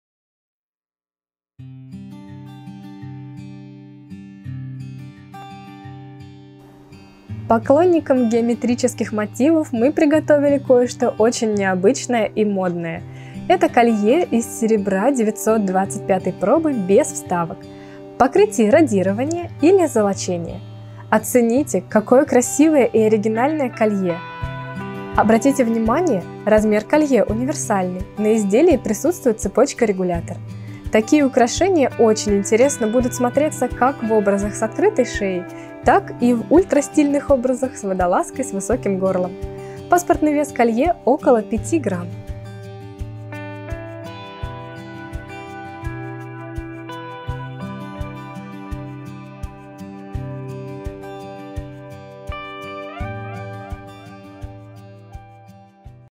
Колье 43012574Д (родирование) и Колье 43012575 (золочение)
Без вставок

Поклонникам геометрических мотивов мы приготовили кое-что очень необычное и модное! Оцените, какое красивое и оригинальное колье – в покрытии на ваш выбор – родирование либо золочение. Обратите внимание – размер колье универсальный, на изделии присутствует цепочка регулятор. Такие украшения очень интересно будут смотреться как в образах с открытой шеей, так и в ультрастильных образах с водолазкой с высоким горлом!

Параметры изделий:
Паспортный вес колье около 5 грамм.